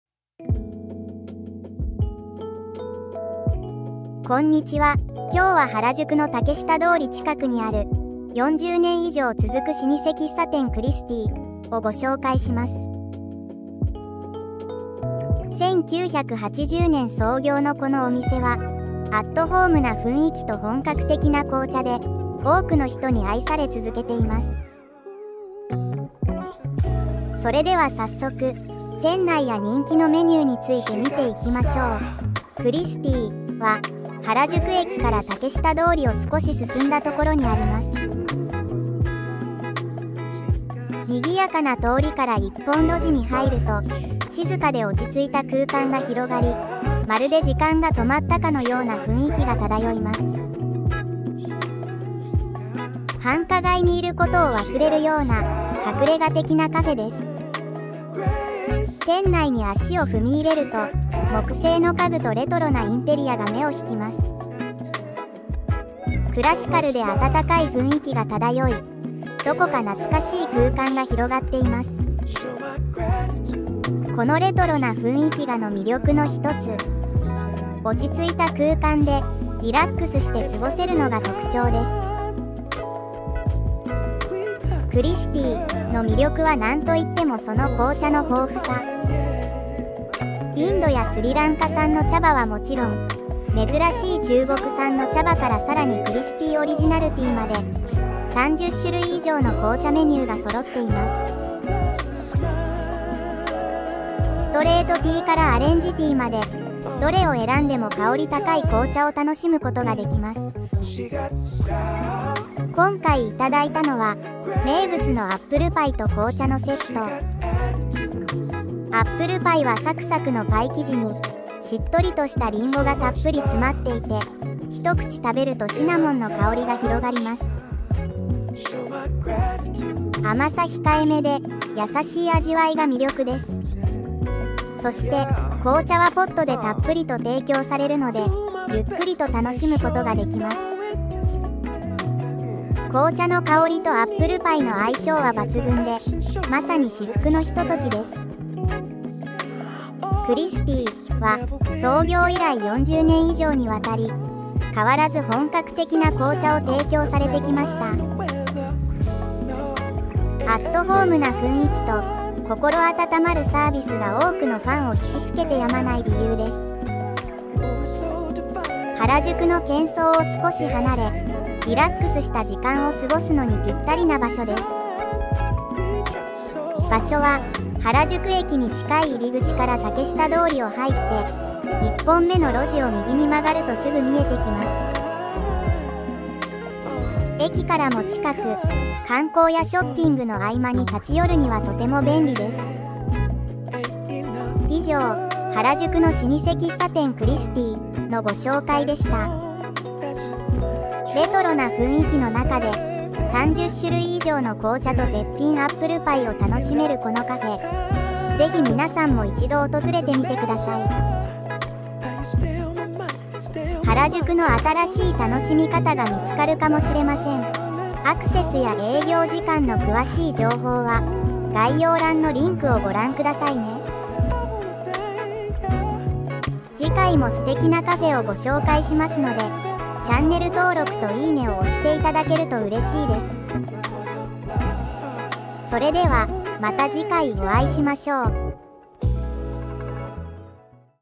【原宿】竹下通り近くで40年以上続く老舗喫茶店「Christie（クリスティ）原宿本店」で楽しむ本格紅茶とアップルパイ
1980年に創業して以来、アットホームな雰囲気と本格的な紅茶を提供し続け、地元の人々や観光客に親しまれてきました。竹下通りを少し進んで1本目の路地を右に曲がると、繁華街にいることを忘れるような静かな空間が現れます。
原宿の中心にありながら、落ち着いてゆっくりとした時間を過ごせる隠れ家的カフェです。